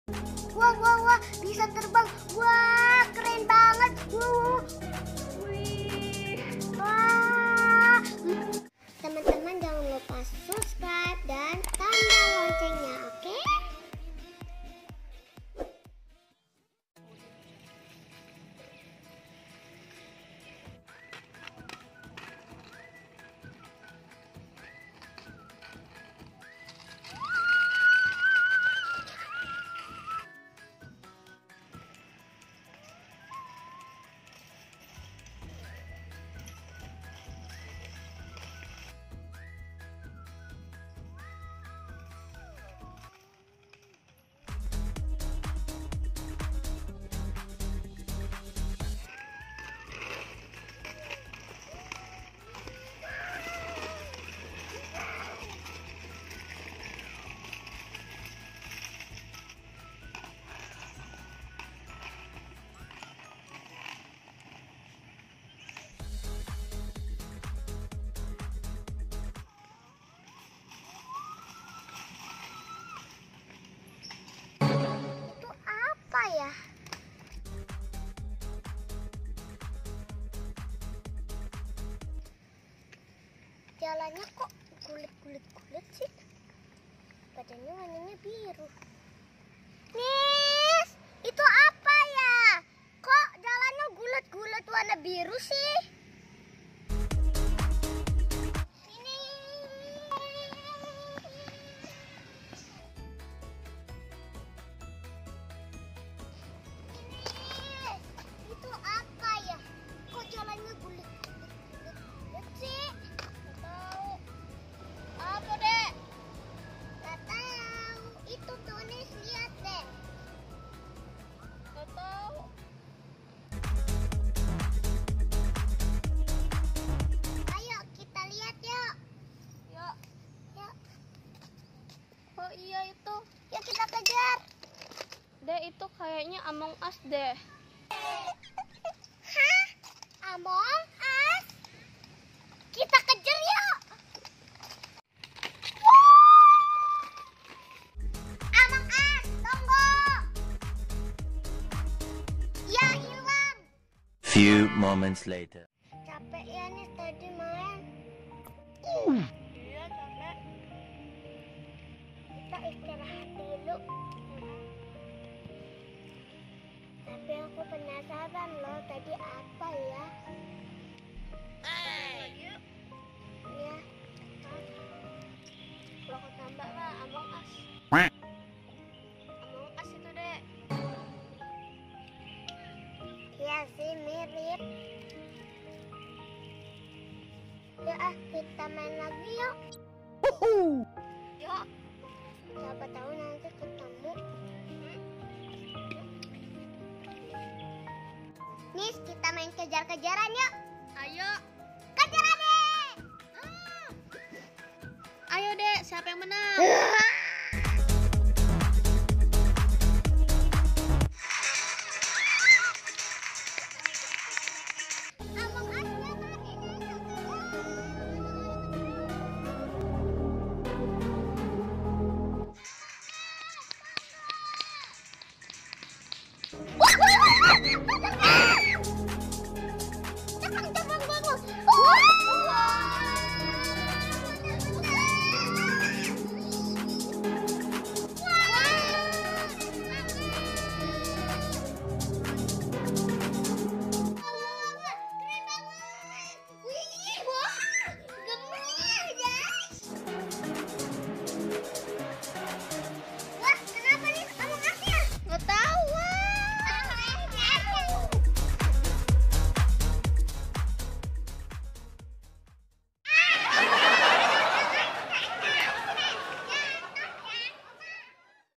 Fakhira bertemu Among As ditaman😱_Drama parodi Among Us | fakhira kibbo
Selamat pagi teman teman...

Alhamdulillah semoga sehat slalu yaaa..❤️

Oh iya di vidio kali ini fakhira buat Drama Parodi Among As lucu banget seruuu...

Terima kasih ya buat kalian yang sudah setia ikutin vidio fakhira..🙏🙏